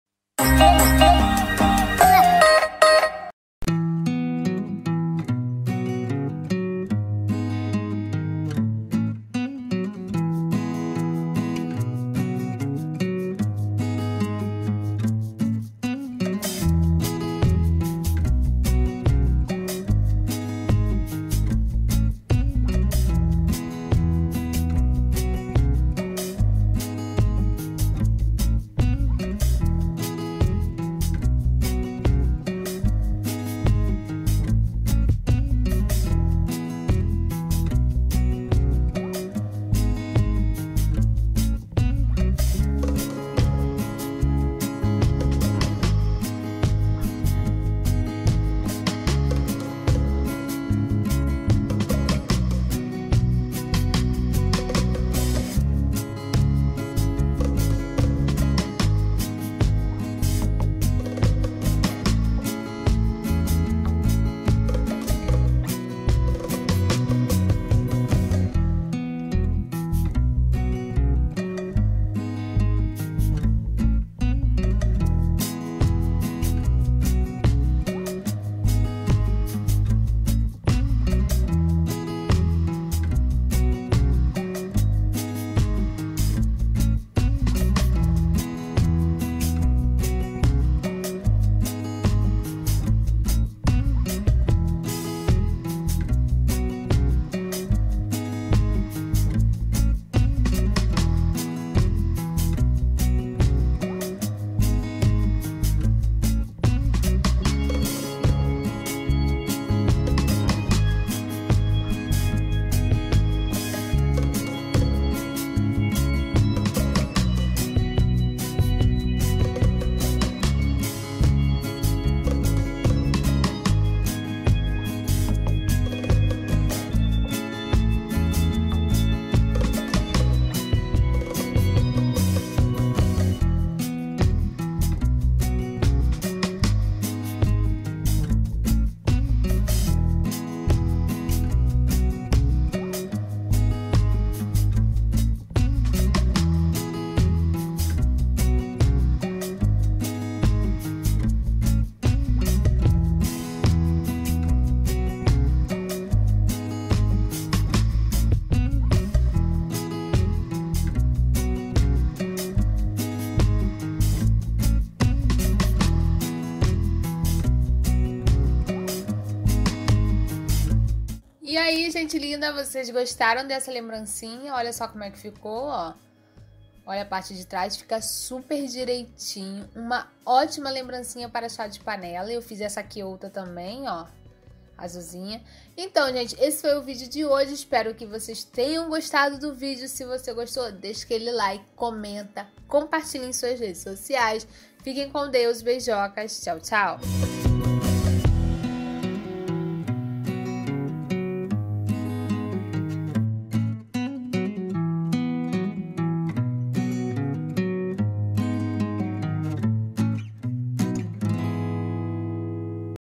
Lembrancinha para Chá de Cozinha fácil de fazer / Chá de Panela ♥ Hellen Chagas
Lembrancinha para Chá de Cozinha fácil de fazer / Chá de Panela

❤ Gostou? CURTA

MATERIAIS:
- EVA
- Tesoura